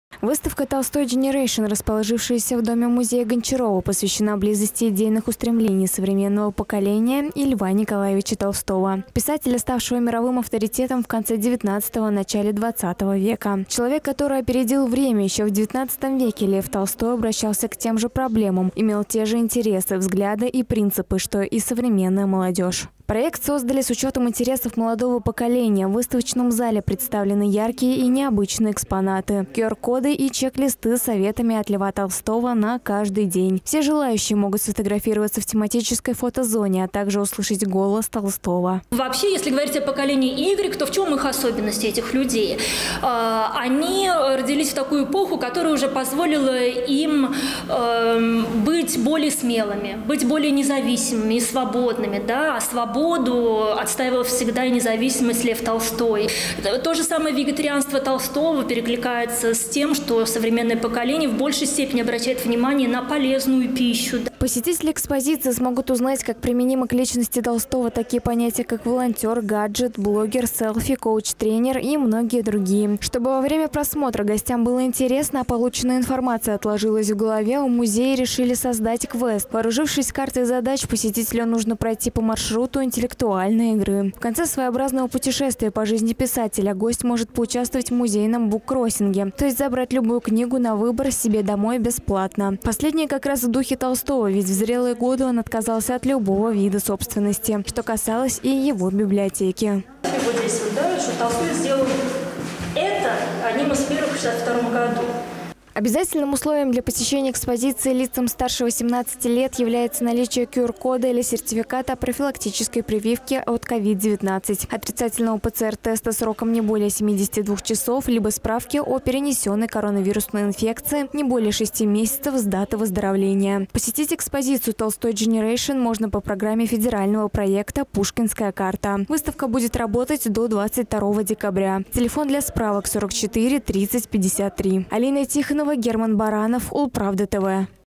TolstoYgeneration вне времени. В музее Гончарова посетители отправляются в квест
Экспозицию предоставил музей усадьба Л.Н. Толстого «Ясная Поляна».
Выставка «TolstoYgeneration», расположившаяся в доме-музее Гончарова, посвящена писателю, ставшему мировым авторитетом в конце XIX – начале ХХ вв. Человеку, который опередил время. Ещё в XIX веке Лев Толстой обращался к тем же проблемам, имел те же интересы, взгляды и принципы, что и нынешнее поколение.